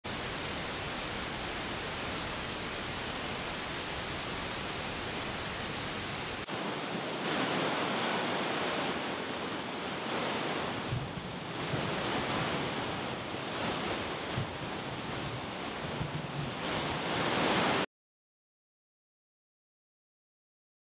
La rivière des Outaouais se déchaîne
Les vagues frappaient vigoureusement les terrains de Zibi, le dimanche 28 avril dernier.